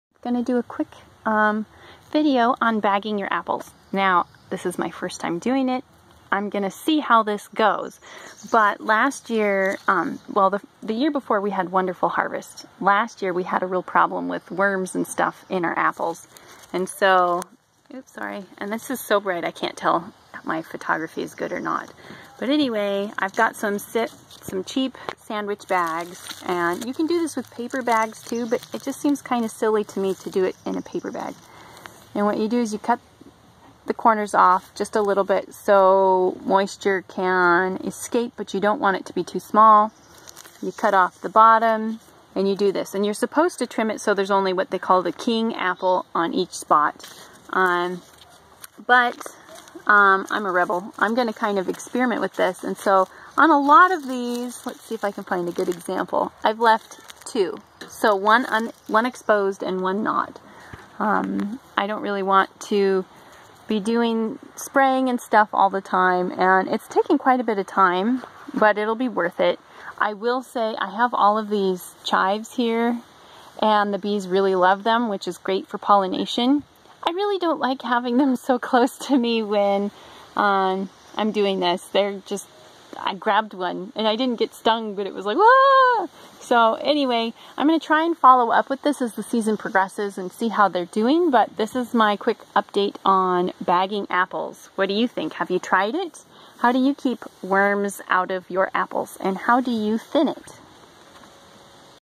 Bagging Apples- how to prevent worms from getting your fruit
Bagging apples to keep the worms from getting my fruit!
You can bag with paper bags, but that is harder to get a better seal, can disintegrate as it's weathered, and you need to remove it a week before harvest so fruit can "blush".  The pros with the plastic is its easier to handle, don't need to remove for the blush, and POSSIBLY reusable. We will see this year! Stick with me as we'll be doing updates throughout the year!